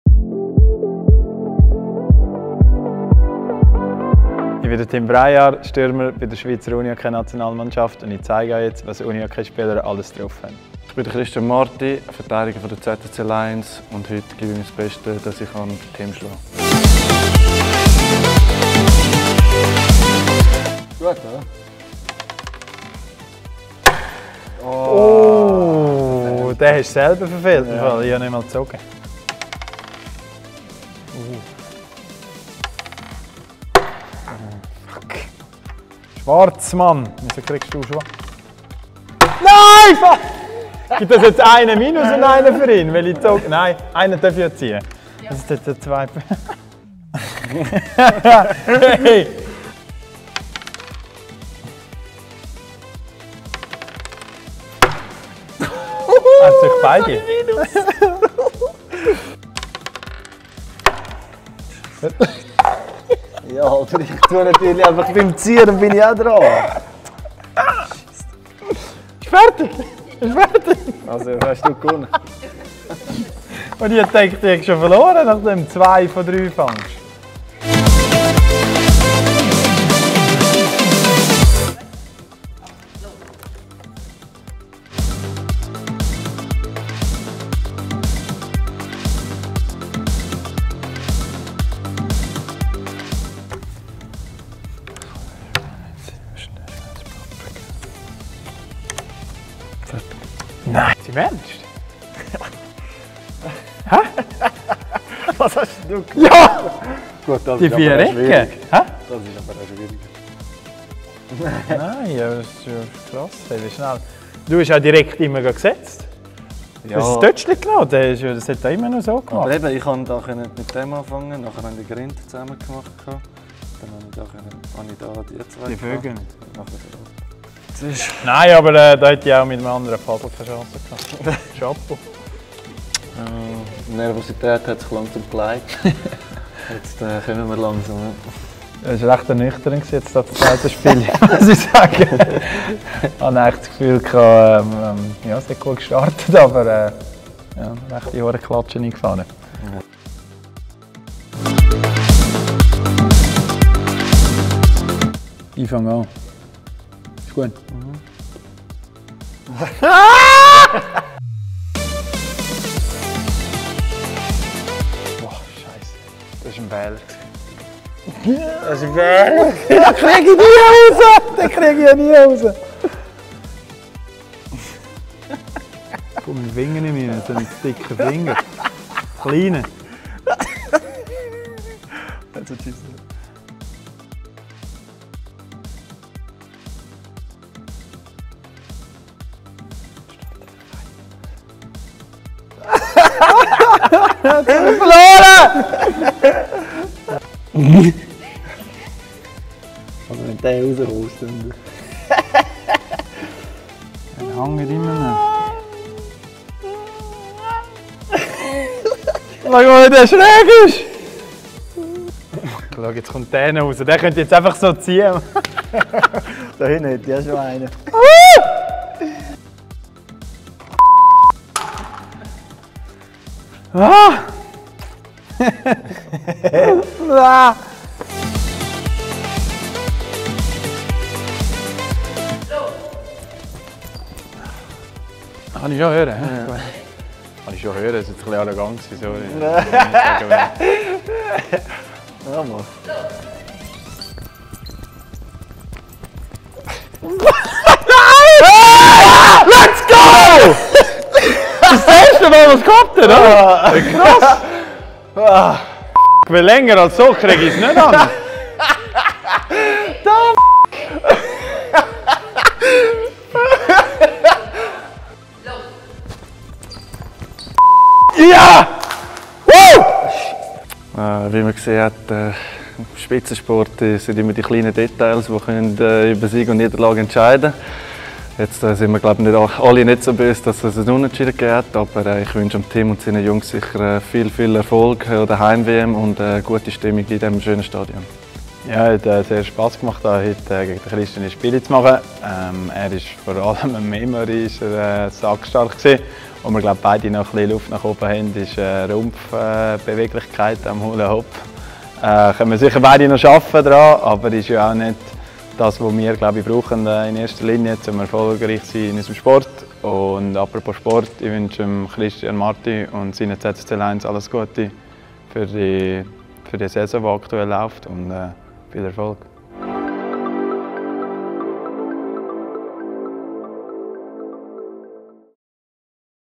Unihockey vs. Eishockey - Wer hat mehr drauf?
Am Samstag finden in der Swiss Life Arena, der neuen Heimstätte der ZSC Lions, die ersten WM-Spiele statt. Doch zuvor wollten wir wissen, wer mehr drauf hat? Tim Braillard, unser Nati-Stürmer oder Christian Marti, Verteidiger bei den ZSC Lions.